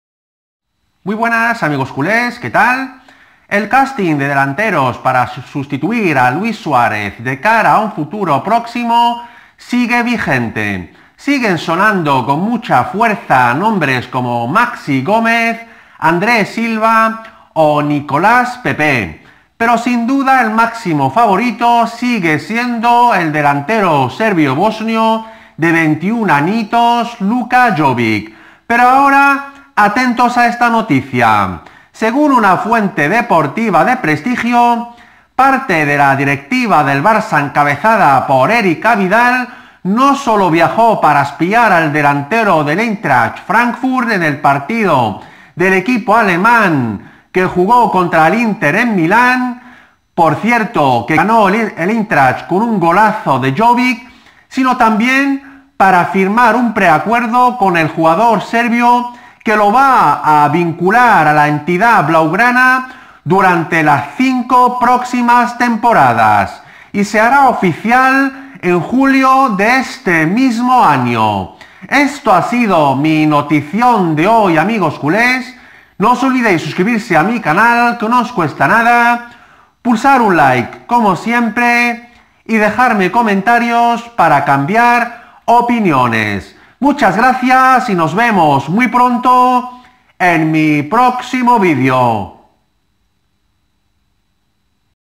ÚLTIMA HORA: LUKA JOVIC FIRMA CON EL BARCELONA
El Barcelona ata al delantero serbio bosnio luka jovic durante las cinco próximas temporadas